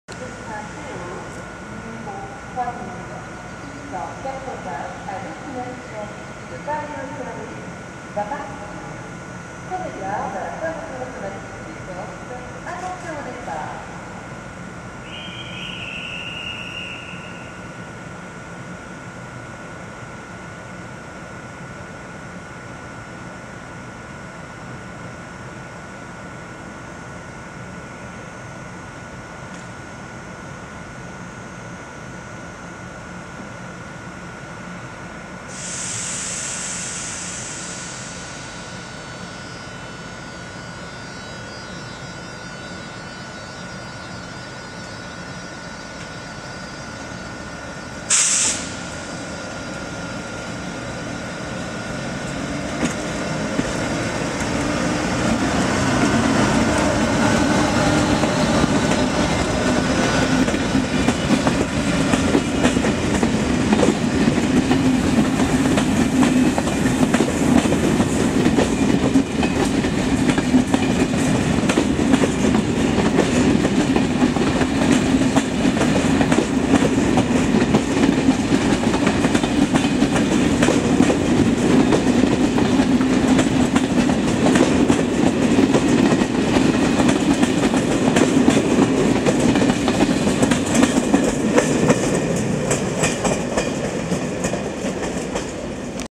Annonce+Départ Téoz avec BB 26005 "En Voyage"
Départ en fin d'après-midi de Brive du Corail Téoz n°3680 à destination de Paris Austerlitz tracté par la Sybic 26005 EV sous un ciel bien gris